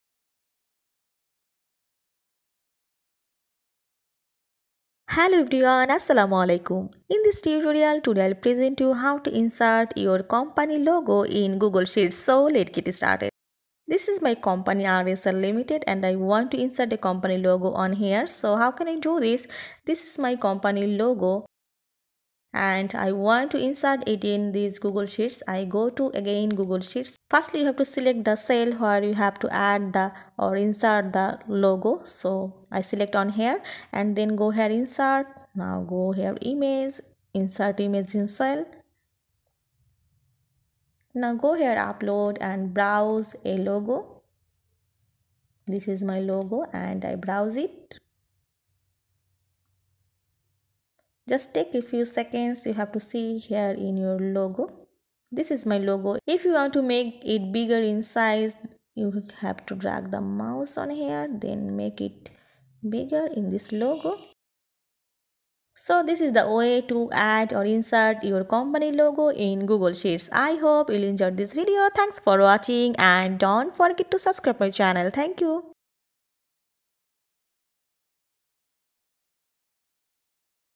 How to insert Company Logo in Google Sheets Easily
Hello everyone, Today I will present you, How to insert Company Logo in Google Sheets Easily.